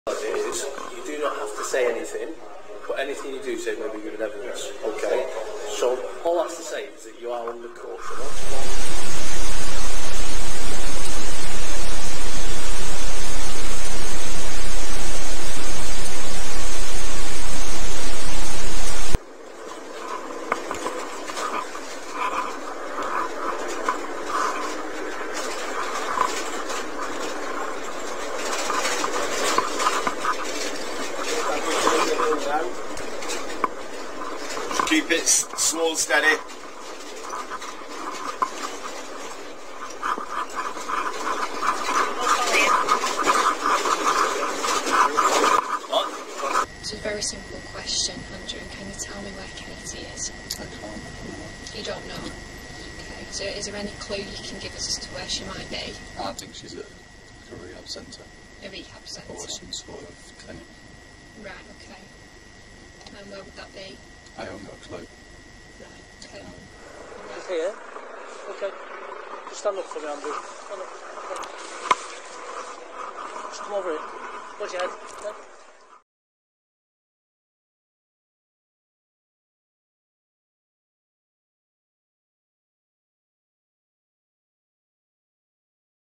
Katie Kenyon's murderer leads police to forest where mother-of-two’s body was buried
Murderer returned to forest to show police where mother-of-two’s body buried

A murderer who buried his ex-girlfriend in a makeshift grave returned to the scene to show police where her body was after claiming he had killed her accidentally.